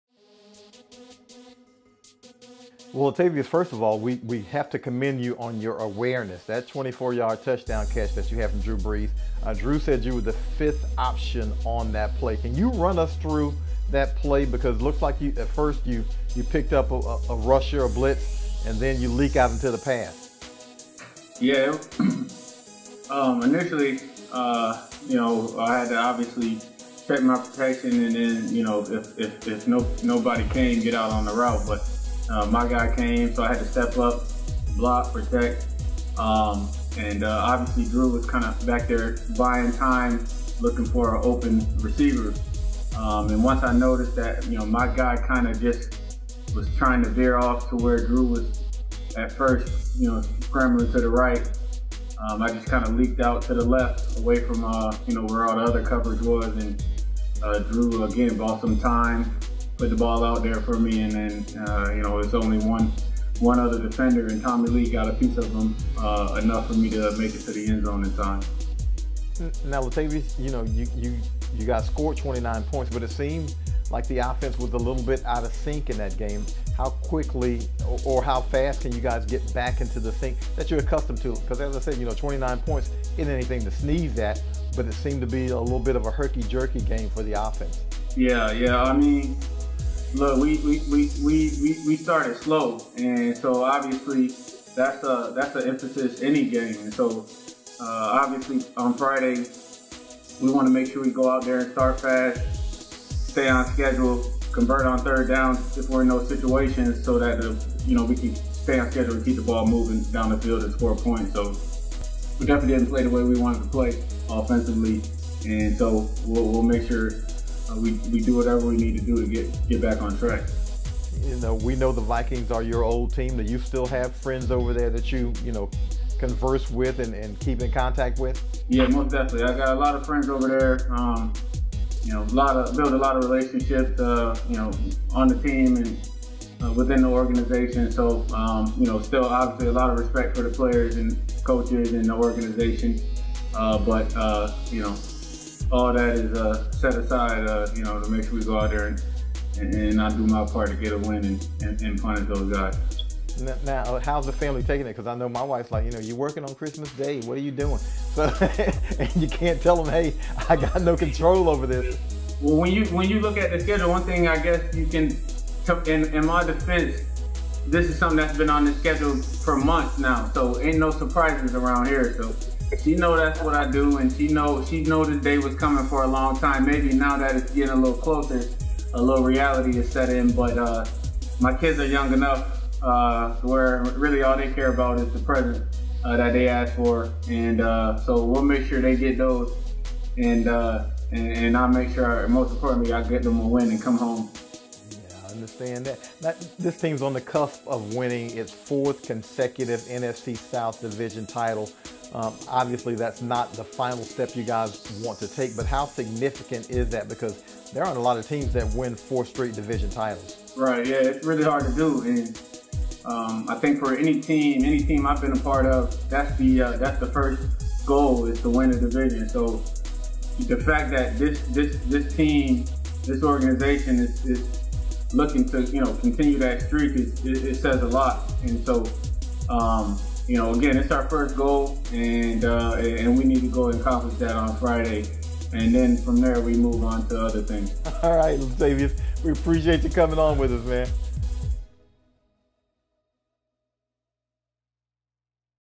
Latavius Murray on Saints-Vikings Week 16 Matchup | Player Spotlight
New Orleans Saints running back Latavius Murray sits down with John DeShazier for a 1-on-1 interview as the team heads into their Week 16 battle against the Minnesota Vikings.